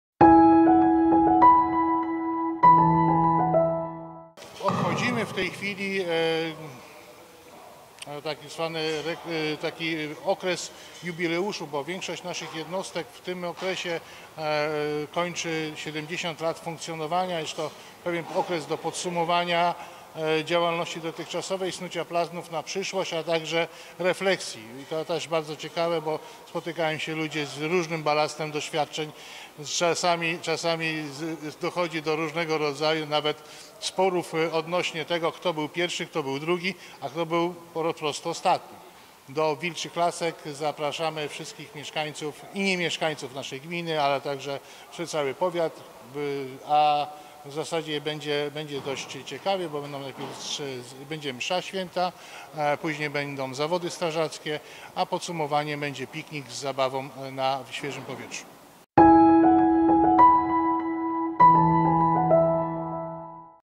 Zaproszenie na jubileusz OSP Wilcze Laski oraz zawody strażackie!
Już w sobotę, 28 maja w Wilczych Laskach (gm. Szczecinek) odbędą się uroczyste obchody 75-lecia istnienia jednostki OSP Wilcze laski połączone z zawodami i piknikiem strażackim. Na mieszkańców regionu będzie czekać wiele atrakcji to m.in .pokazy lotnicze, przysmaki lokalnych kół gospodyń wiejskich, ukraińskie specjały oraz gry i zabawy ze szczecineckim SAPiKiem. Nie zabraknie stoisk lokalnych firm, przedsiębiorstw, nadleśnictw, a także jednostek OSP.

Już w sobotę, 28 maja w Wilczych Laskach! Rozpoczynamy mszą świętą w kościele o godzinie 11:00, o godzinie 12:00 obchody 75-lecia istnienia OSP Wilcze Laski!

Zapraszamy!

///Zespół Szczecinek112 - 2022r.